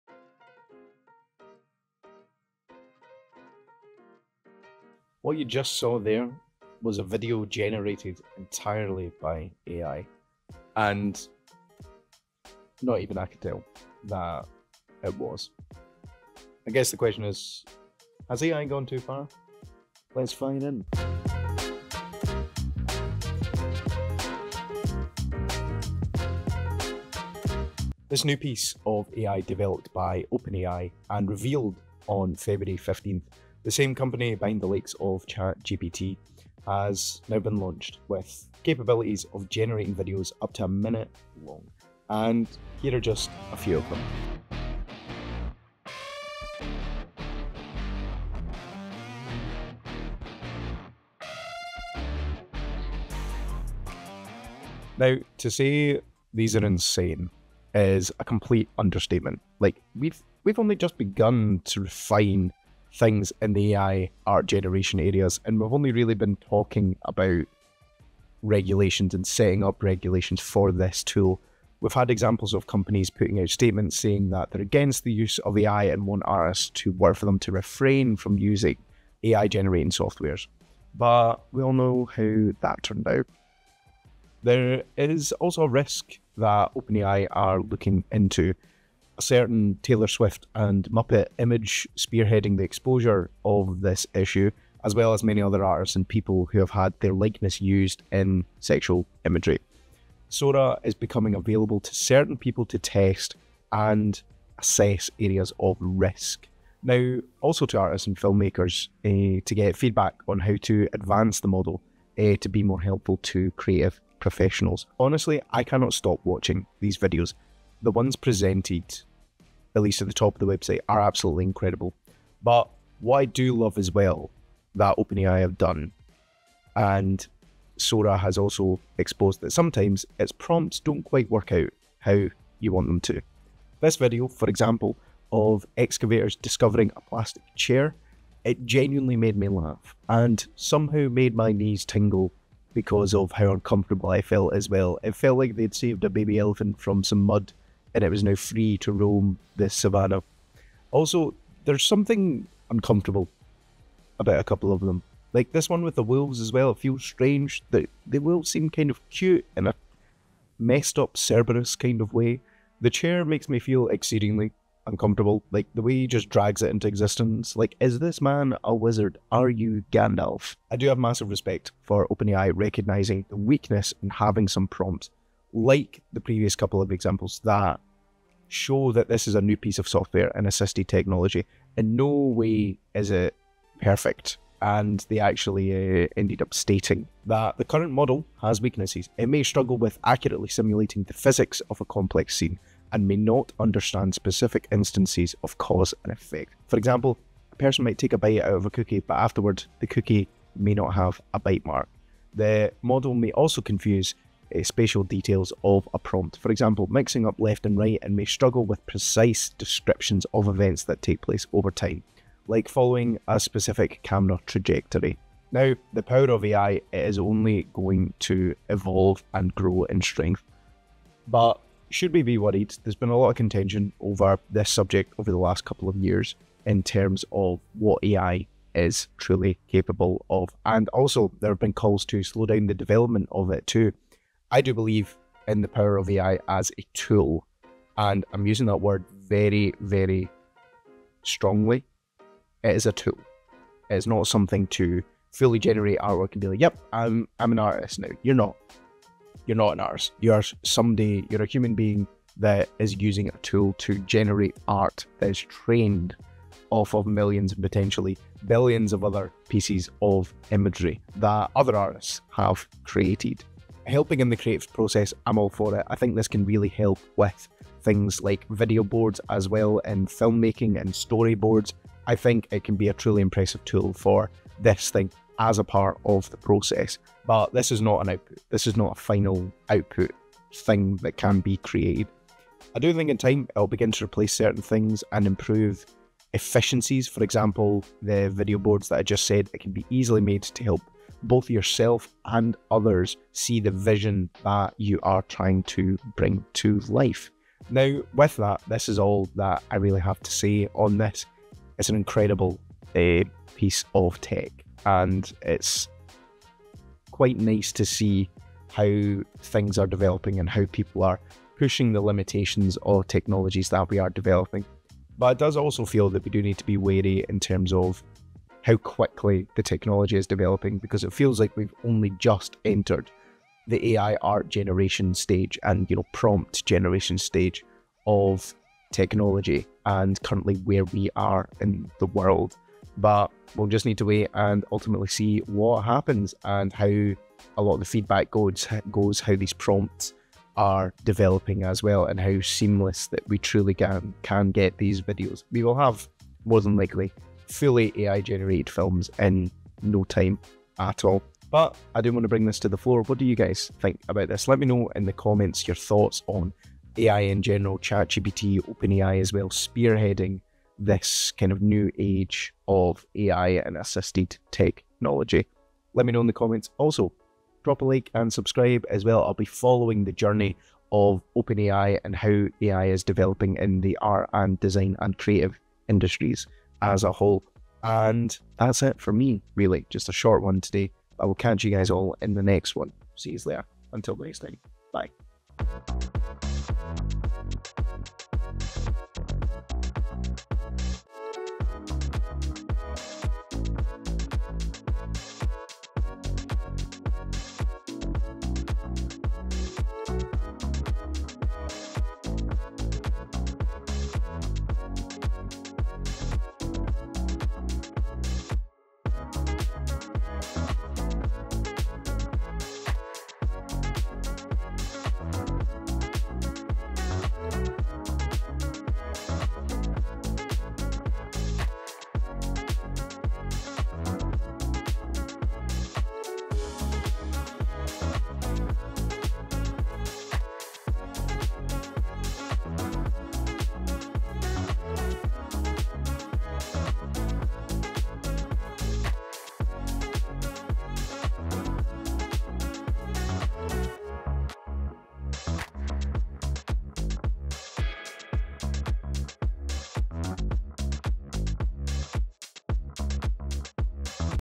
This Changes Everything : Sora AI
We're barely a year through developments in the AI space and Sora has been launched to certain creators to test and give feedback.